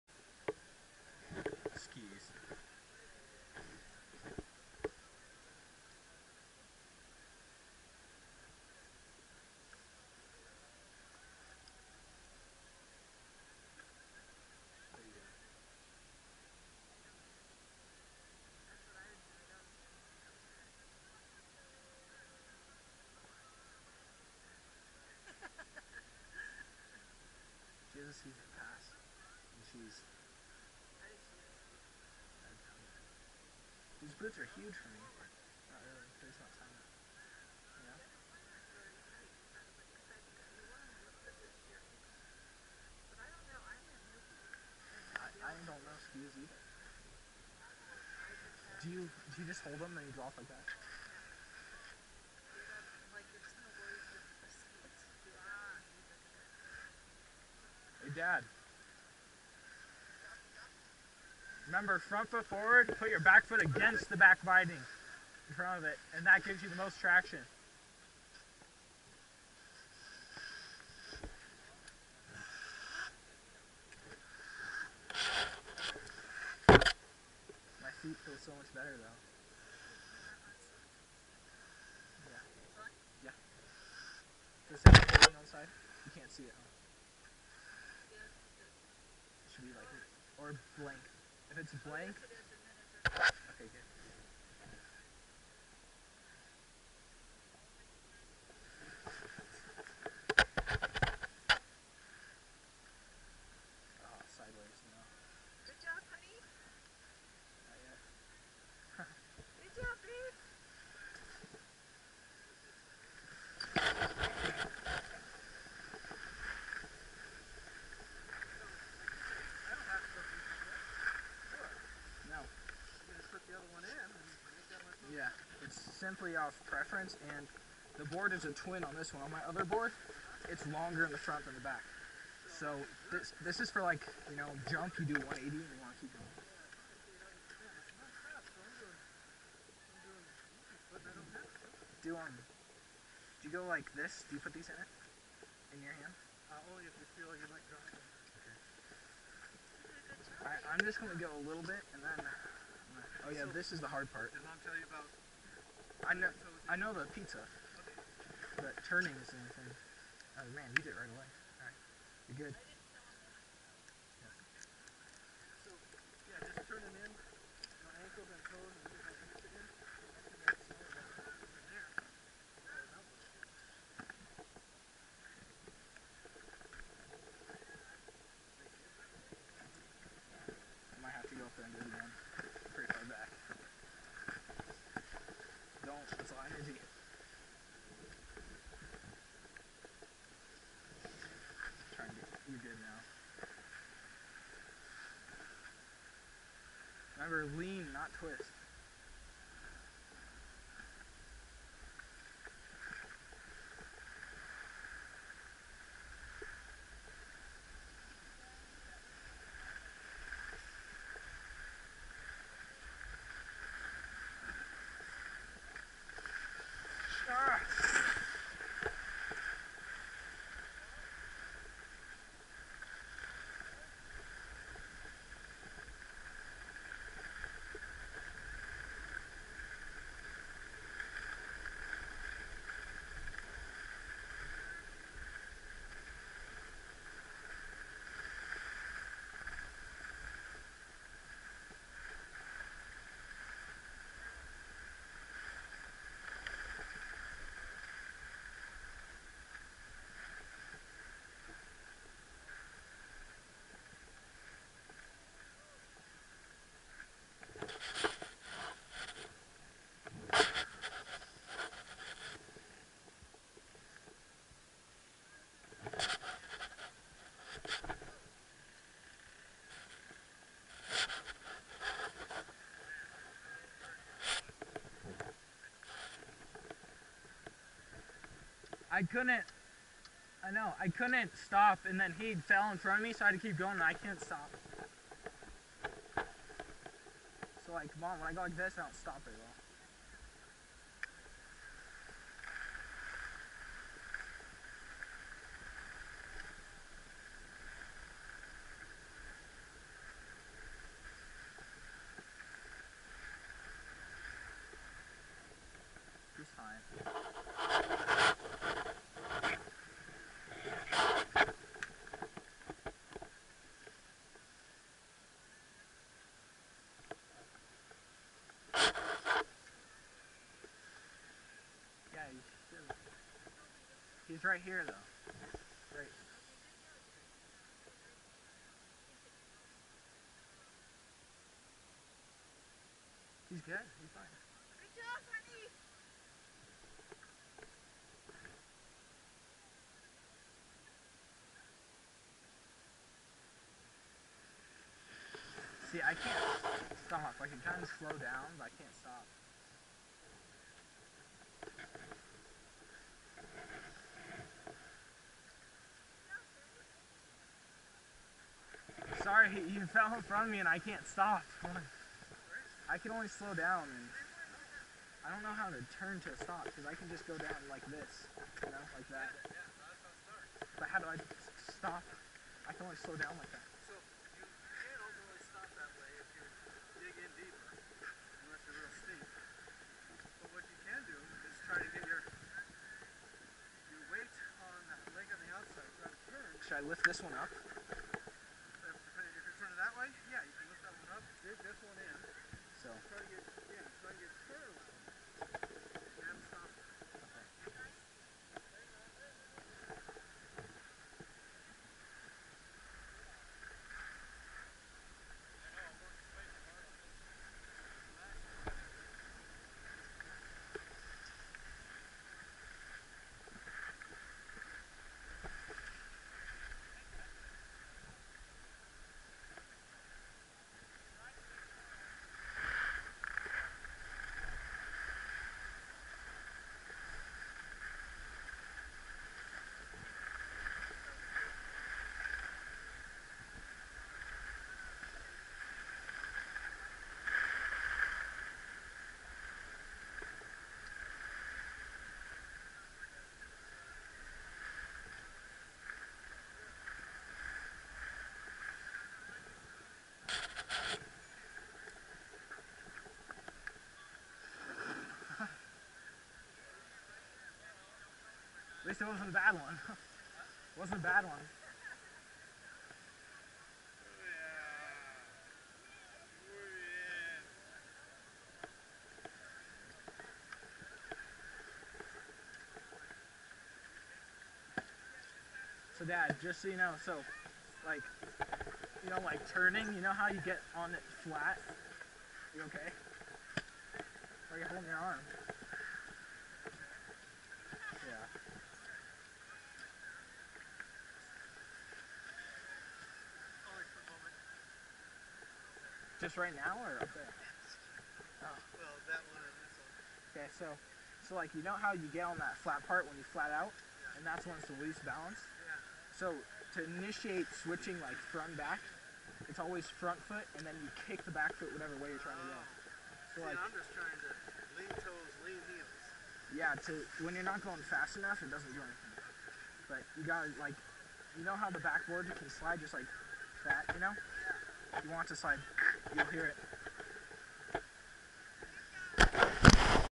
Attempting to Ski on the Bunny Hill at Northstar
Sony Action Cam HDRAS-15 at 1080p 30fps